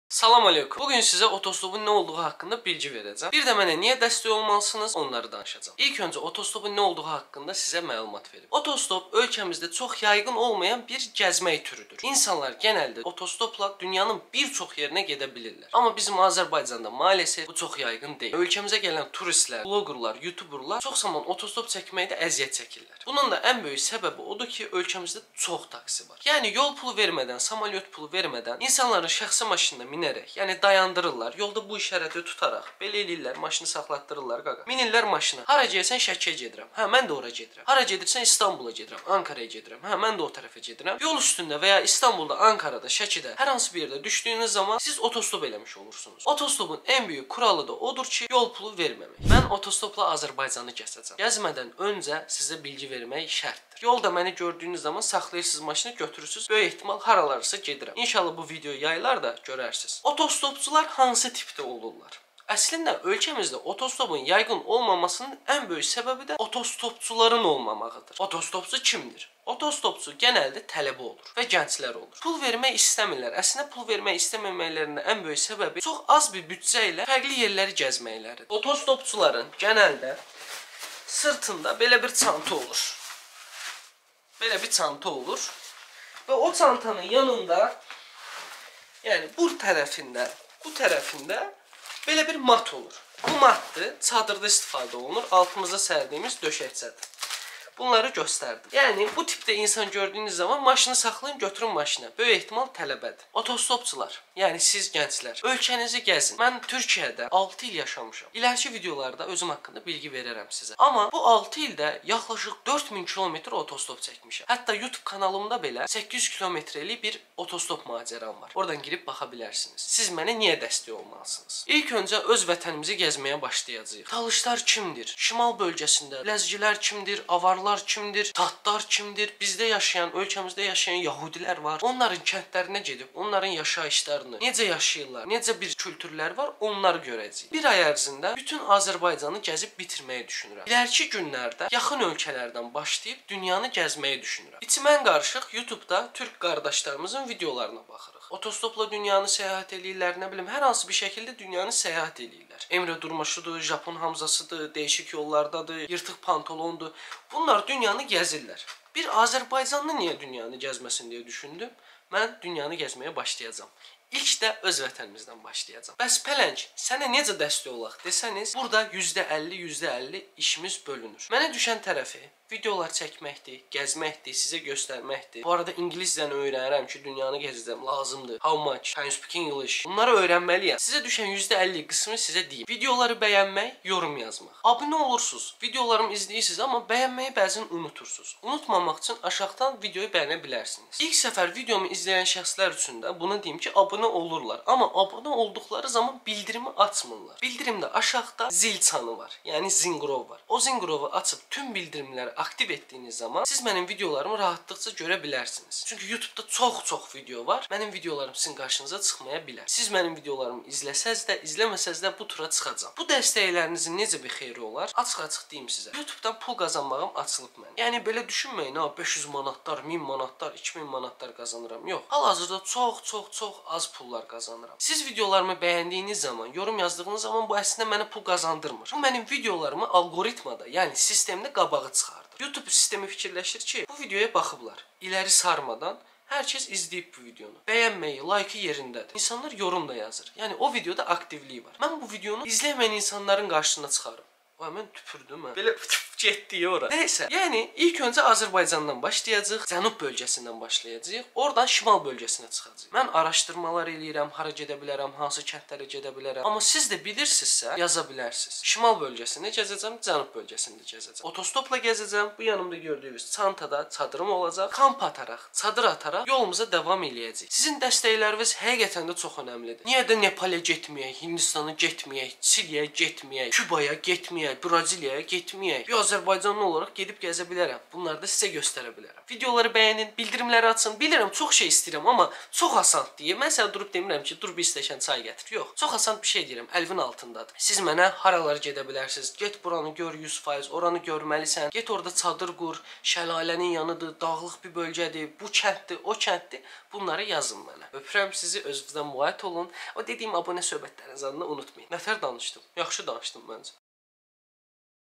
OTOSTOP NƏDİR ? Mənə Niyə Dəstək Olmalısız ? (DÜNYA TURU)
Azerbaycanda otostop ! Otostop Nədir ? Bu videomda hamısını açıqladım size. Yaxın zamanda Azərbayanda Otostopla bir çox yerini birlikte gəzəcik !

Diğer videolarımı izlemek isterseniz;